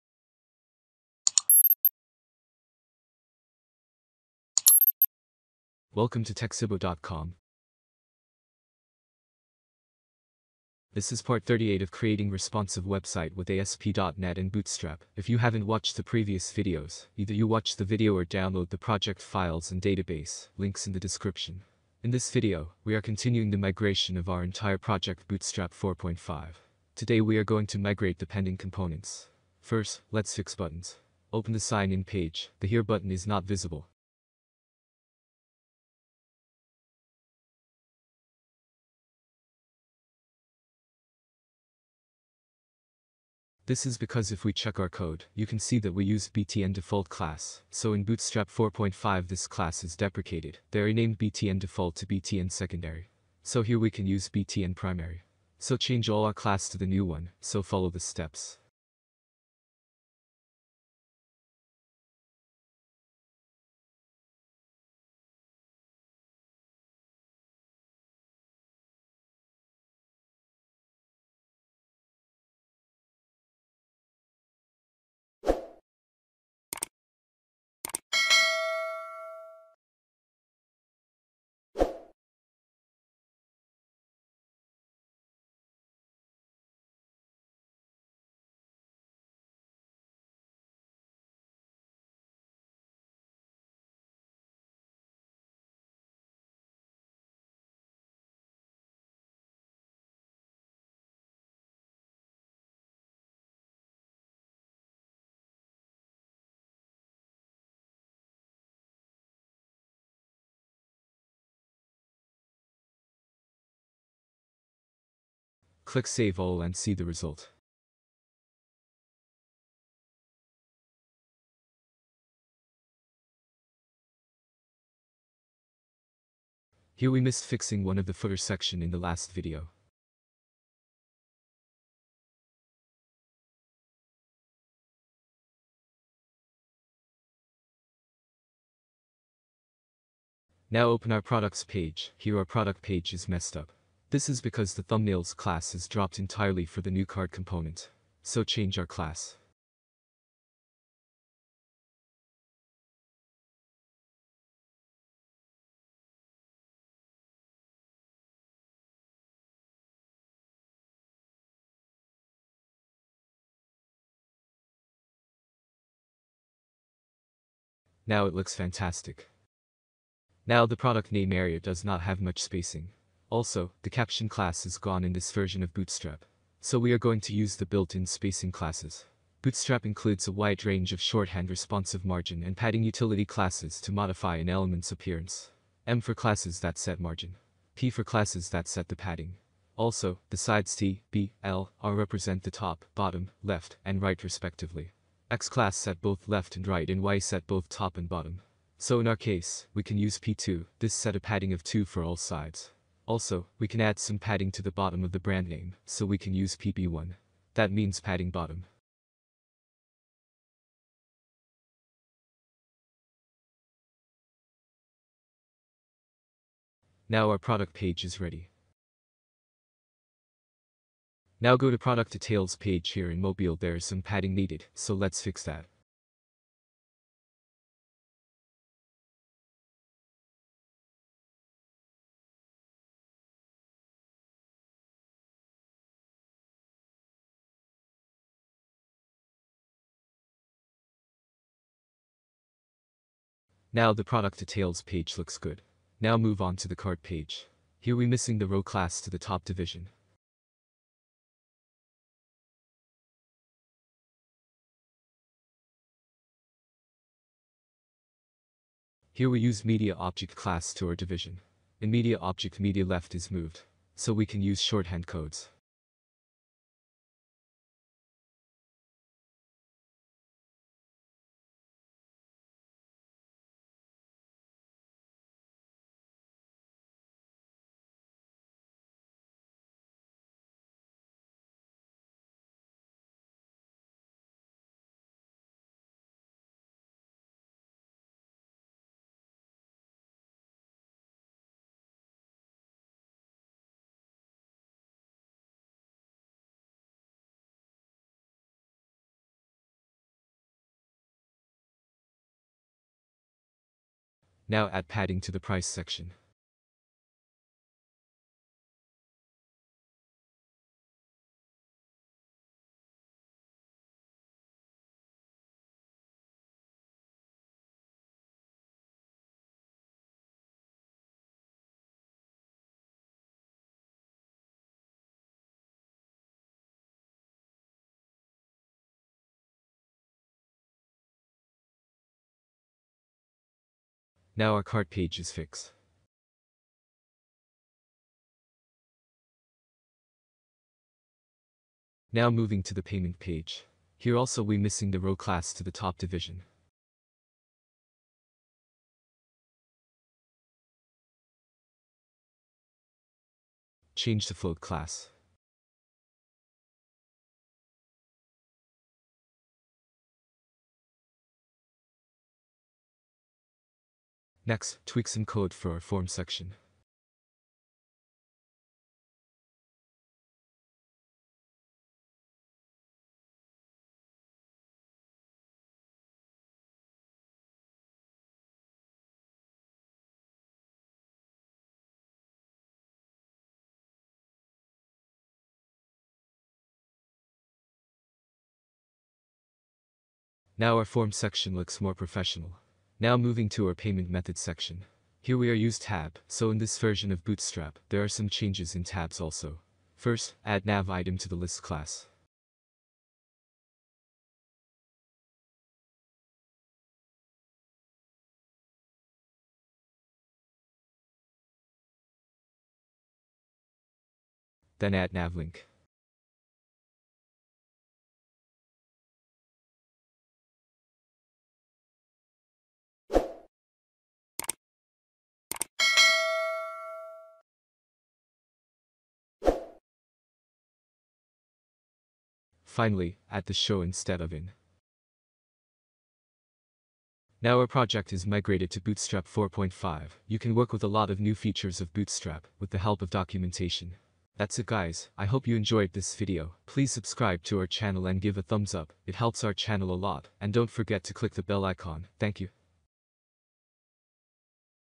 Responsive Website-ASP.NET&Bootstrap-Part 38-Online Shopping Site-Bootstrap 4.5 Spacing,Buttons
This video explains how to migrate bootstrap version 3 to bootstrap version 4.5, in this video, I will explain about spacing that gives shortcodes of different purposes also buttons.